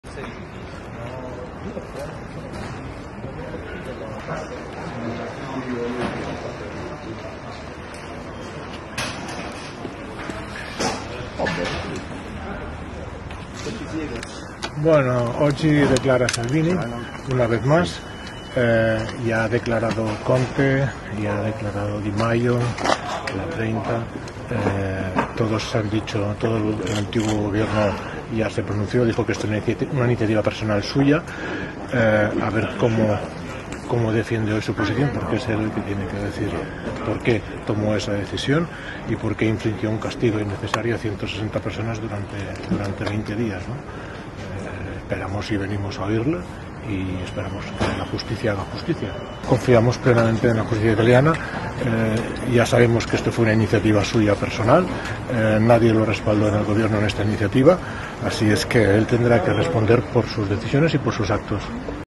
Open Arms, Oscar Camps: "Sappiamo che negare sbarco fu decisione di Salvini"
(LaPresse) "Siamo qui davanti all'aula bunker a Palermo per il processo contro il ministro Salvini che oggi farà delle dichiarazioni: già avevano testimoniato Conte e Toninelli, Di Maio, Trenta, tutto l'ex governo ha dichiarato e constatato che fu una sua decisione personale di infliggere questo castigo a 160 persone, privandole dell'assistenza necessaria e tenendole in mare. Aspettiamo di sentire la sua versione, già sappiamo che il governo non sostenne la sua decisione e che fu una sua decisione personale". Così Oscar Camps, fondatore della ong Open Arms, in una dichiarazione video poco prima dell'inizio dell'udienza di oggi a Palermo dove verrà sentito Matteo Salvini, accusato di sequestro di persona e rifiuto d'atti d'ufficio per aver impedito, quando era ministro dell'Interno, lo sbarco dei migranti salvati dalla nave dell'ong spagnola. "Questo - aggiunge Camps - sta diventando più chiaro. Abbiamo piena fiducia nella giustizia italiana".